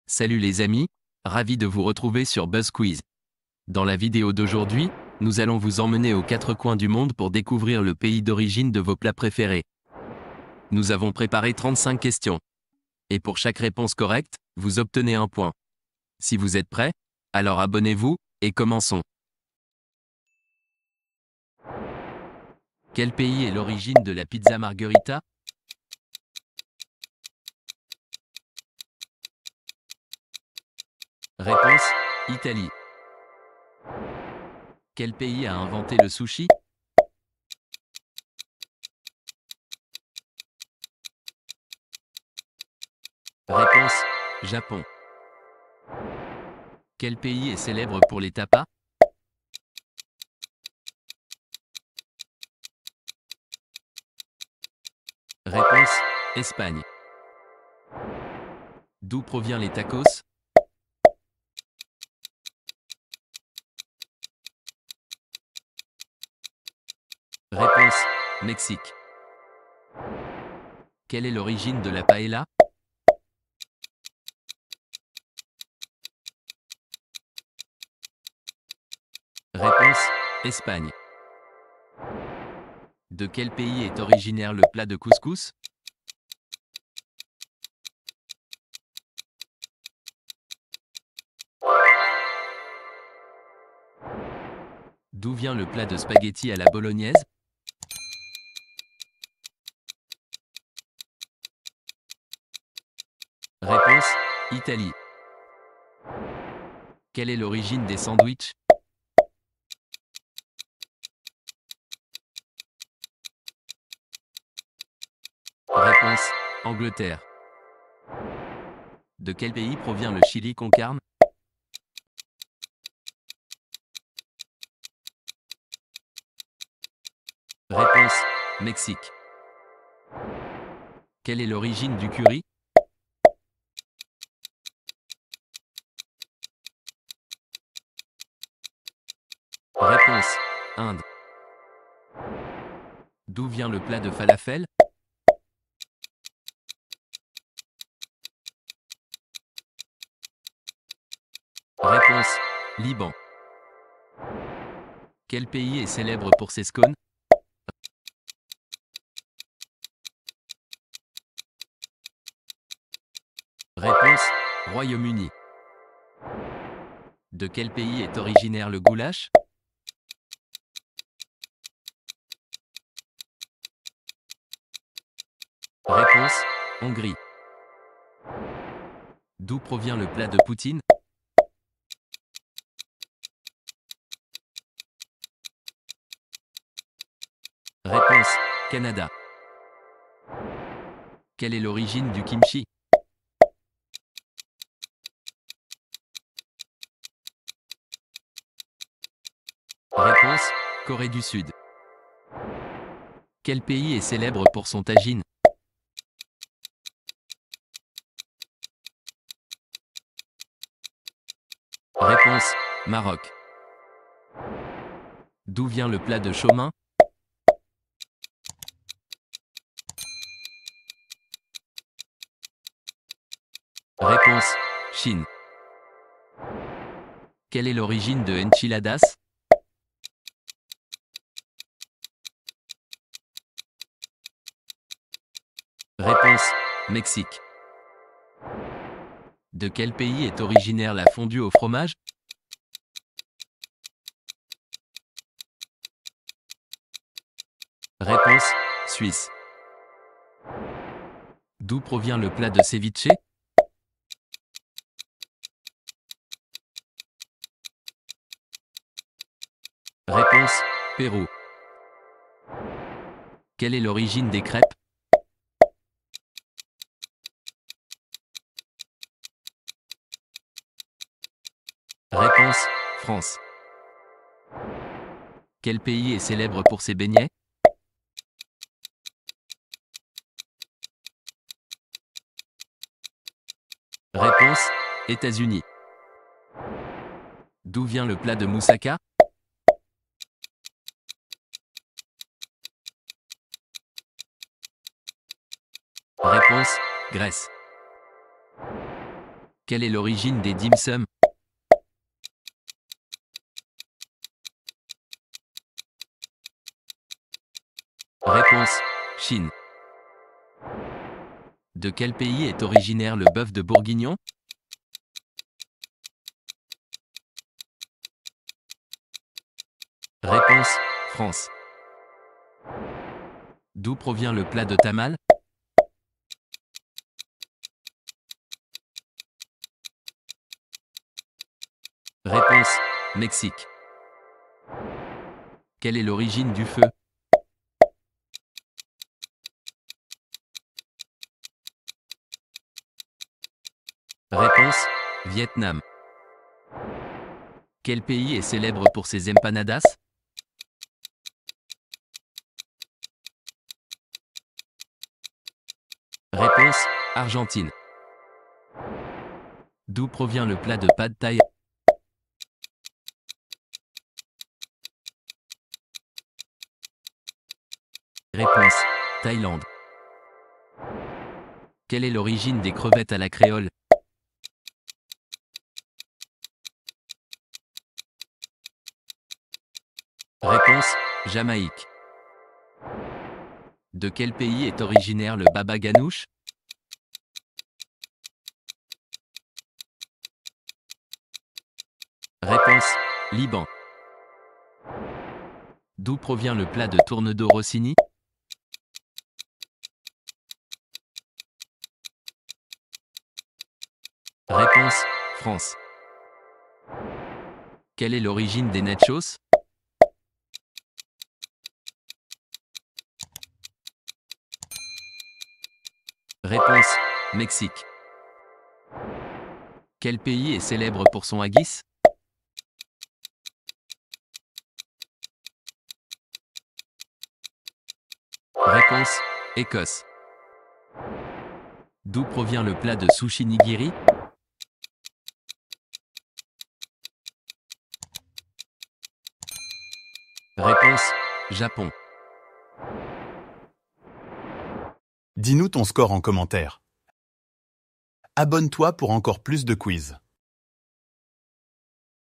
Quiz de Cuisine en 35 Questions ! 🍳 | Testez vos connaissances 🧠 et Dévoilez vos talent Culinaire
Êtes-vous prêt à devenir un véritable chef en cuisine ? Notre Quiz de Cuisine en 35 Questions est là pour mettre vos compétences culinaires à l'épreuve ! Découvrez des faits fascinants sur la gastronomie, les techniques de cuisson et les plats du monde entier. Que vous soyez un amateur passionné ou un expert en cuisine, ce défi stimulant est conçu pour éveiller votre palais et enrichir vos connaissances. Préparez-vous à impressionner vos amis à la prochaine réunion !